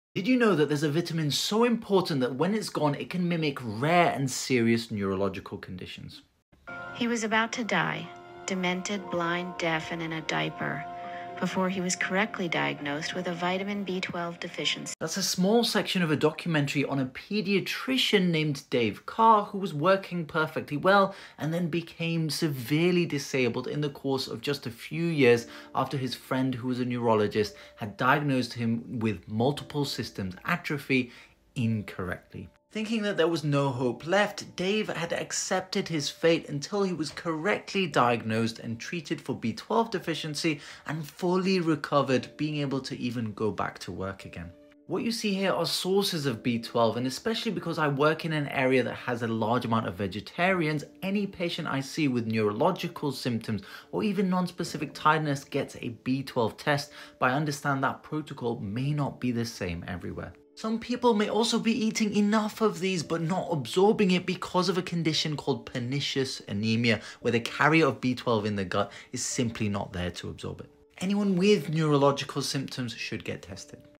I’m Sure It’s That”
Vitamin B12 deficiency can be surprisingly serious, especially when it comes to the nervous system. Without enough B12, the body struggles to make healthy red blood cells and to maintain the protective covering around nerves. Over time, this can lead to problems like numbness and tingling in the hands or feet, memory difficulties, mood changes, unsteady walking, or even confusion.
What makes B12 deficiency tricky is that its symptoms can look like other rare neurological diseases. For example, it may resemble multiple sclerosis, certain forms of dementia, or other degenerative conditions. This can delay proper diagnosis if B12 levels aren’t checked. The good news is that, unlike many of those rare diseases, B12 deficiency is treatable—often with supplements or injections—and many symptoms can improve if it’s caught early.